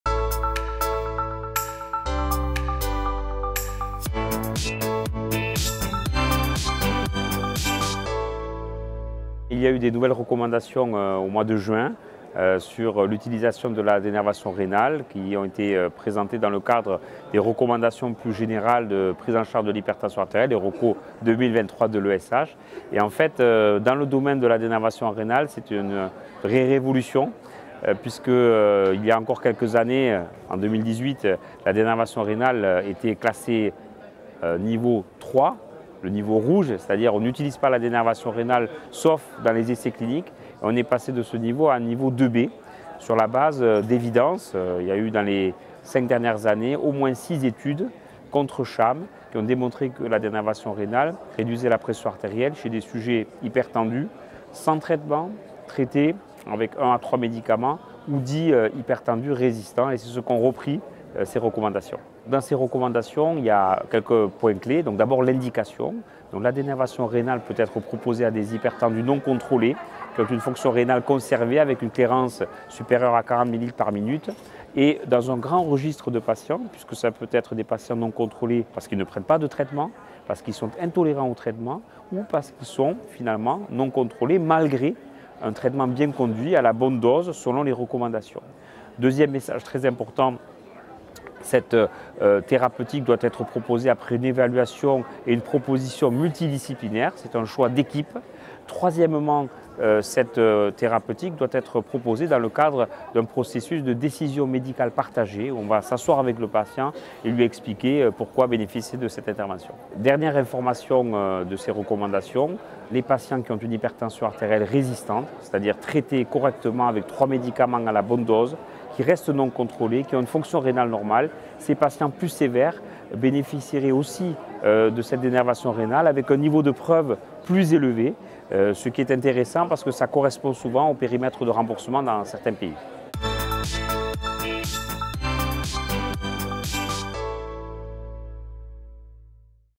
Quelle place pour la dénervation rénale dans les recommandations 2023 de l'ESH ?
La révision de l'indication de la dénervation rénale dans les recommandations ESH 2023 est présentée comme une petite "révolution" par Atul Pathak, qui nous explique que c'est parce qu'il a pu être démontré ces 5 dernières années que cette procédure réduisait la pression artérielle chez des patients hypertendus, qu'ils soient sans traitement, traités, ou dits résistants, qu'elle peut être désormais classée au niveau II B.

Suivez l'interview de notre expert pour connaître tous les détails, et en savoir plus sur les deux autres préconisations de ces recommandations : privilégier les centres spécialisés et une prise de décision collective.